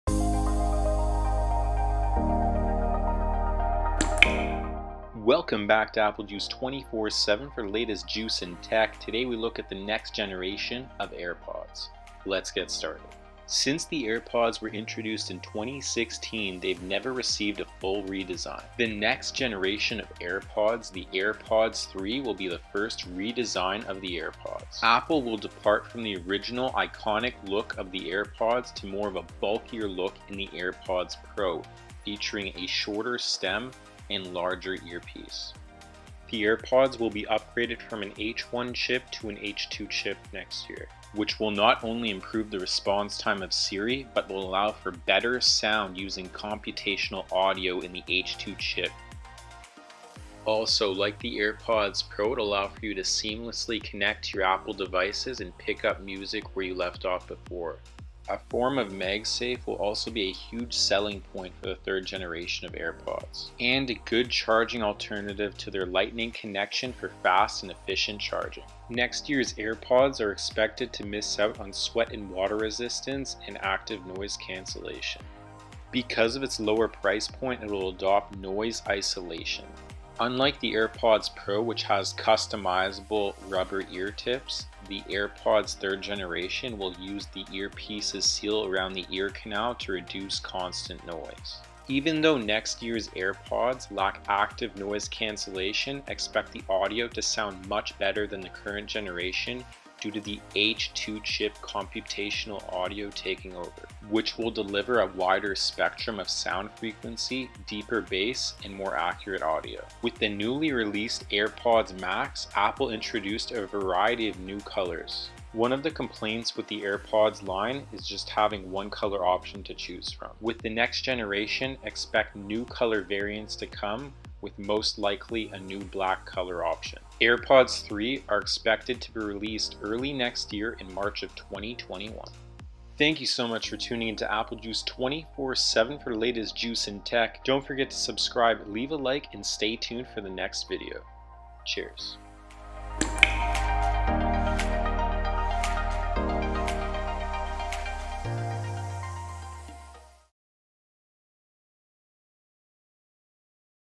AirPods 3: Overview and Rumoured Specs
Full breakdown and features for the 3rd Generation of AirPods. Credit: Apple